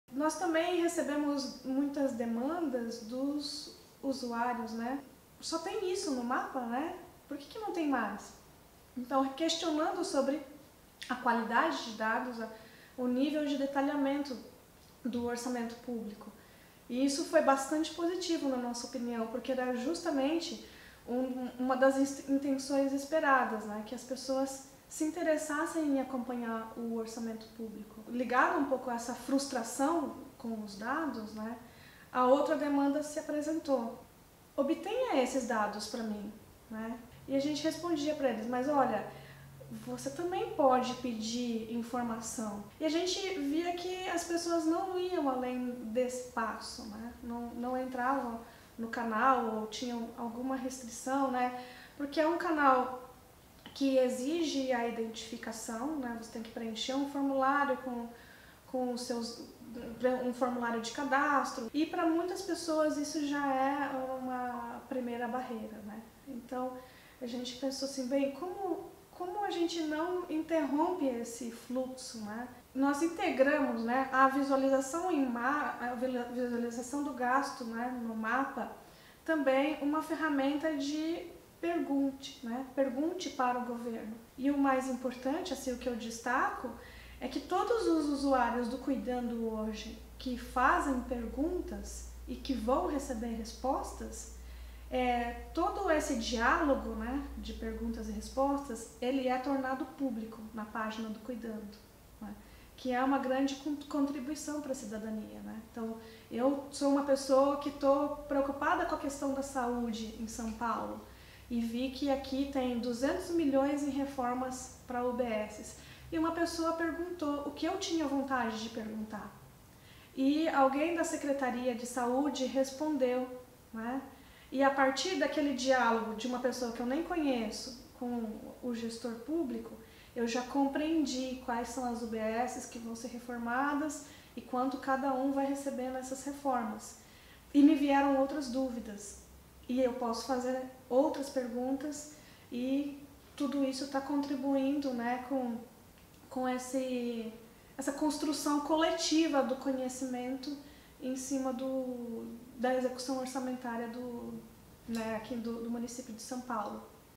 cuidando.vc - Perguntas ao Poder Público
O "Cuidando do Meu Bairro" é uma ferramenta de acompanhamento do orçamento público em tempo real criada na USP-Leste pela profa. Gisele Craveiro e sua equipe. Acesse cuidando.vc e veja como a prefeitura está gastando o seu dinheiro.